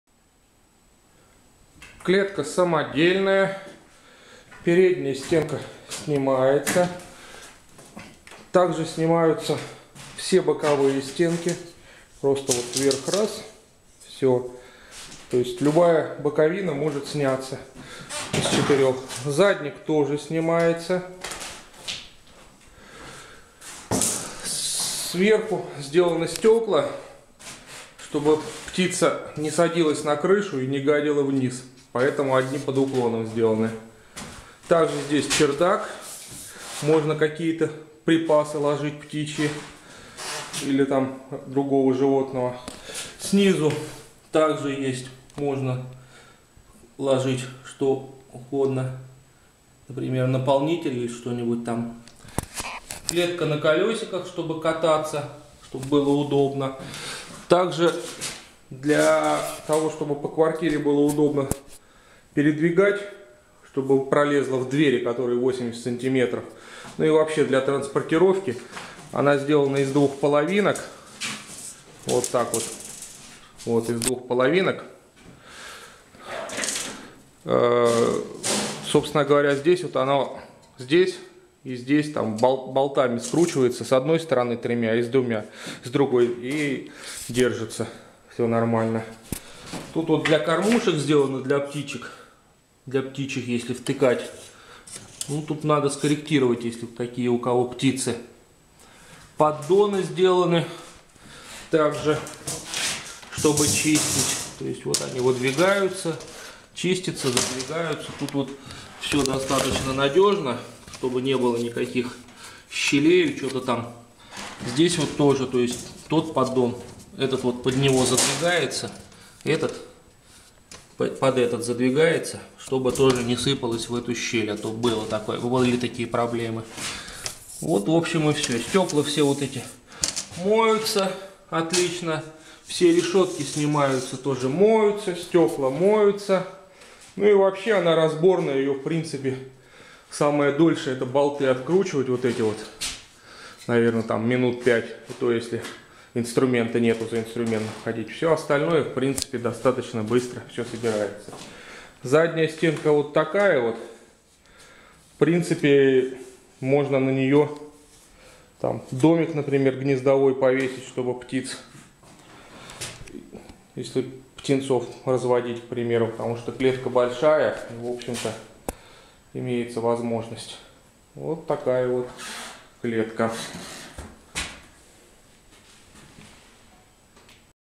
Клетка - Рики 5
Из алюминиевых уголков. Собрана на клепках. Выдвижные поддоны. Разборная. Небольшой вес.